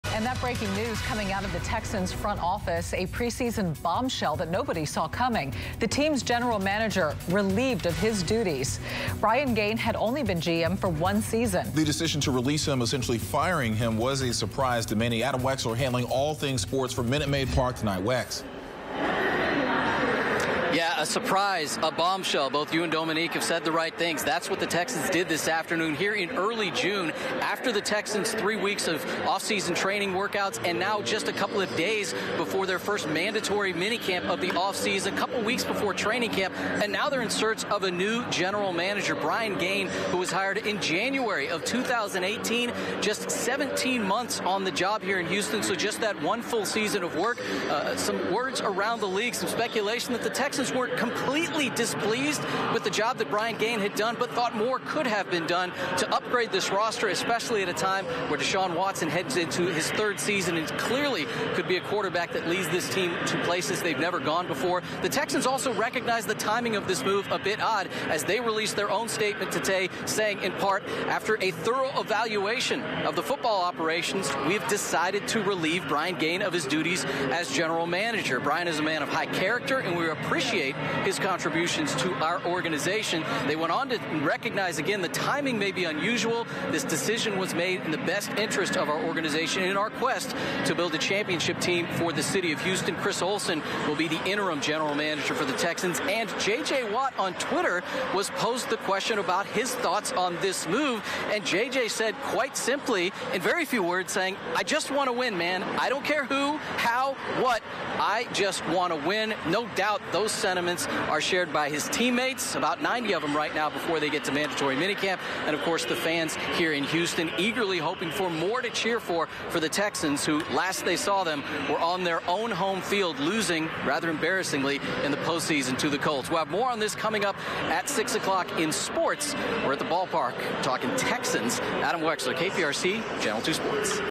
Texans release GM Brian Gaine
Brian Gaine was relieved of his duties with the Texans on Friday, according to Cal McNair.